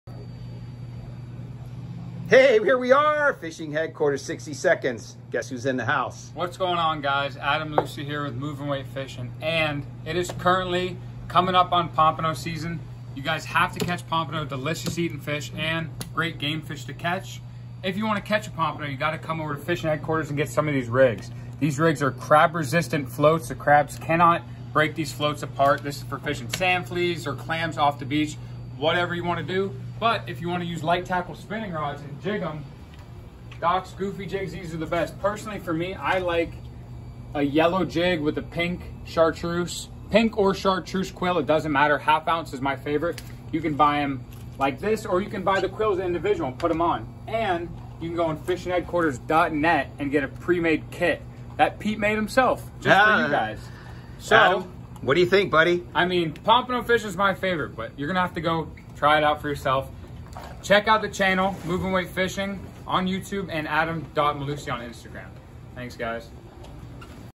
Pompano fishing in Jupiter.
Adam Mellusi stopped by to talk about pompano.